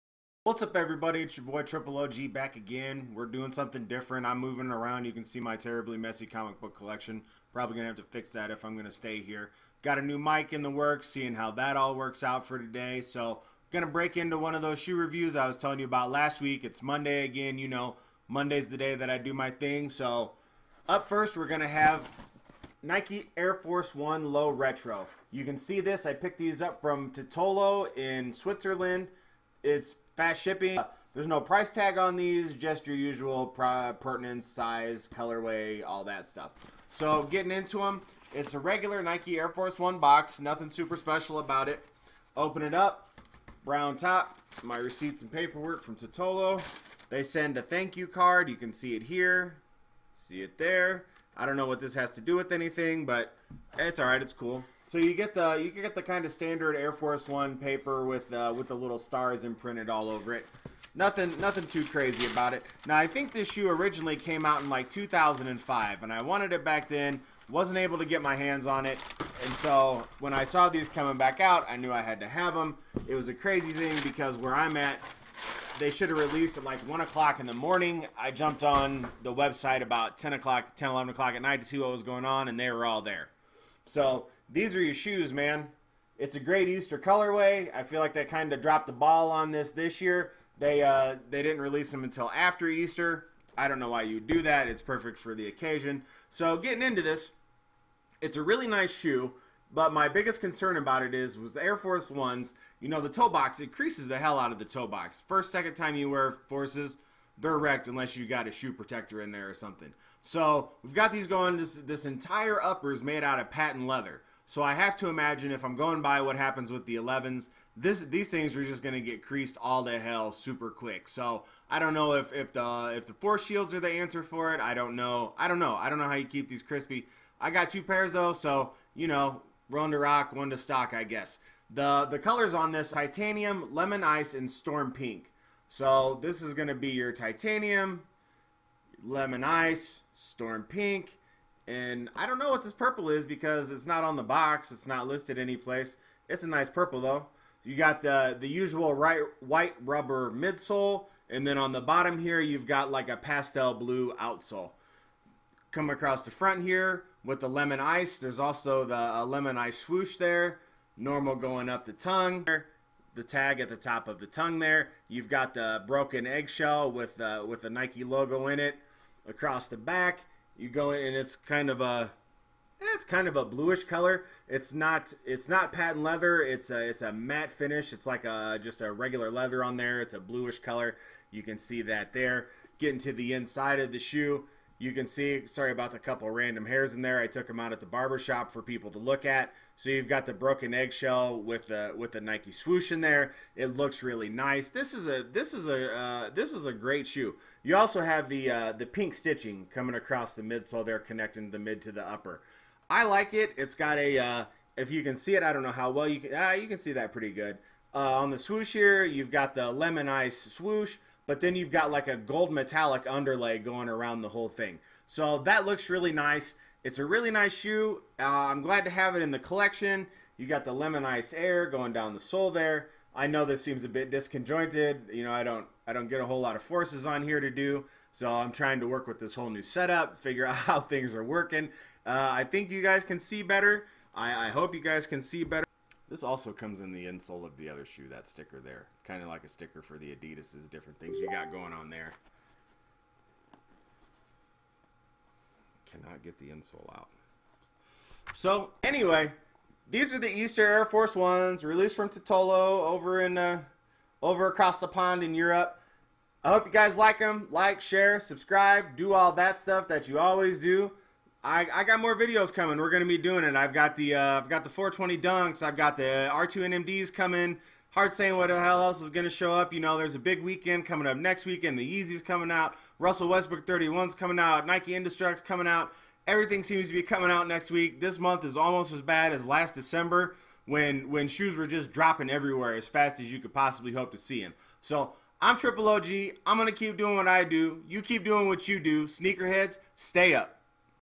Unboxing and Review Nike Air Force 1 "Easter" from Titolo
Unboxing and review of:

NIKE AIR FORCE 1 LOW
RELEASE DATE: APRIL 17TH, 2017
$170
Color: Titanium/Lime Ice-Storm Pink